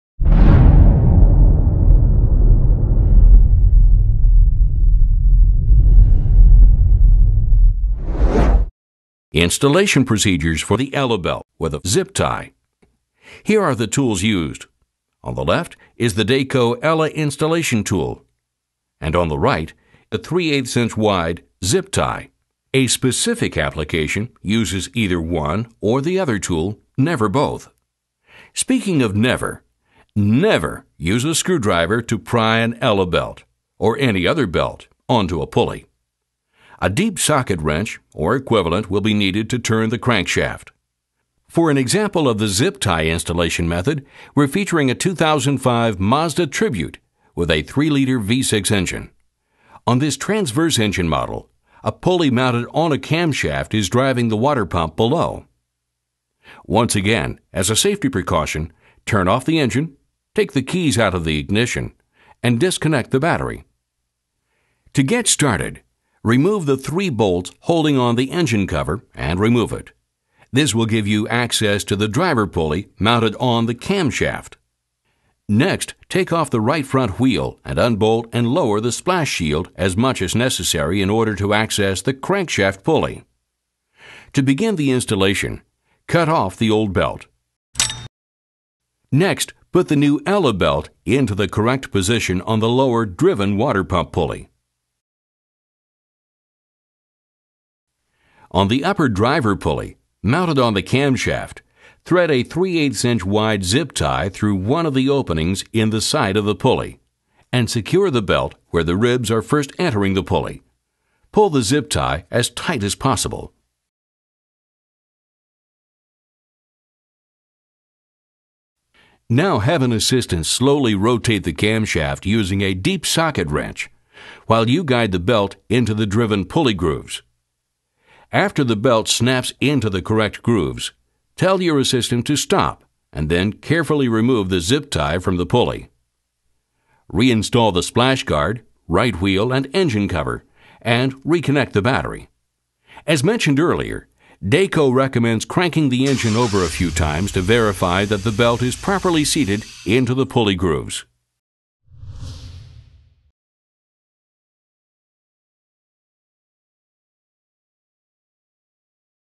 Dayco Installation of the Dayco ela Stretch Belt Using a Zip Tie
In this video, Dayco explains how to install a new ela, or stretch, belt using a 3/8" zip tie.